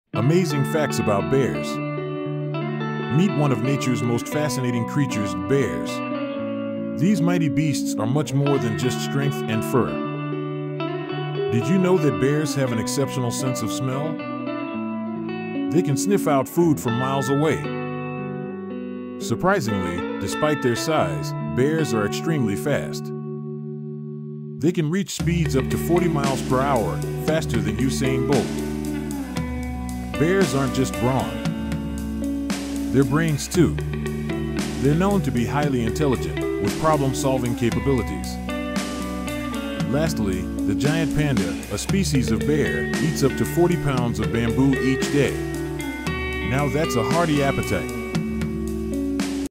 Fun facts about bears
Bears are carnivoran mammals of the family Ursidae. They are classified as caniforms, or doglike carnivorans. Although only eight species of bears are extant, they are widespread, appearing in a wide variety of habitats throughout the Northern Hemisphere and partially in the Southern Hemisphere. Bears are found on the continents of North America, South America, Europe, and Asia. Common characteristics of modern bears include large bodies with stocky legs, long snouts, small rounded ears, shaggy hair, plantigrade paws with five nonretractile claws, and short tails.(Wikipedia)